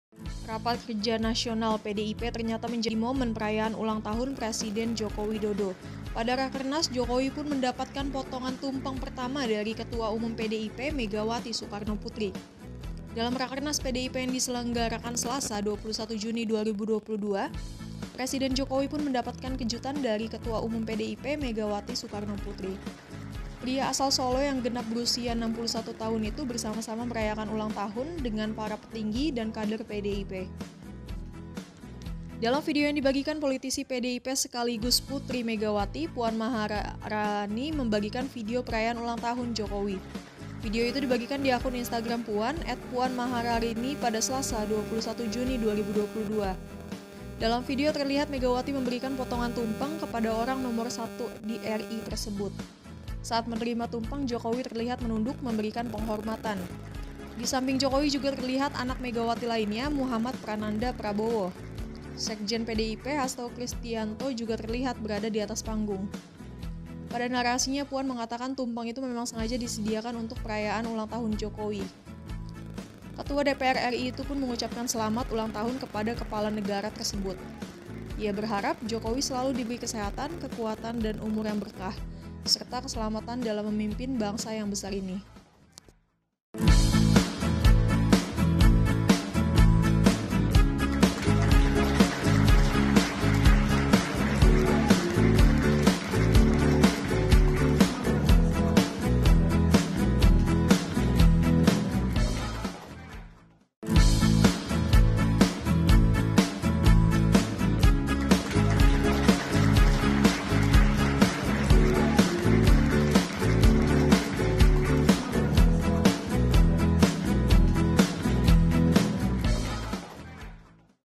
Momen Jokowi Terima Nasi Tumpeng dari Megawati di Hari Ulang Tahun
WARTAKOTALIVE.COM, JAKARTA - Hari Rakernas PDIP ternyata menjadi momen perayaan ulang tahun Presiden Joko Widodo (Jokowi).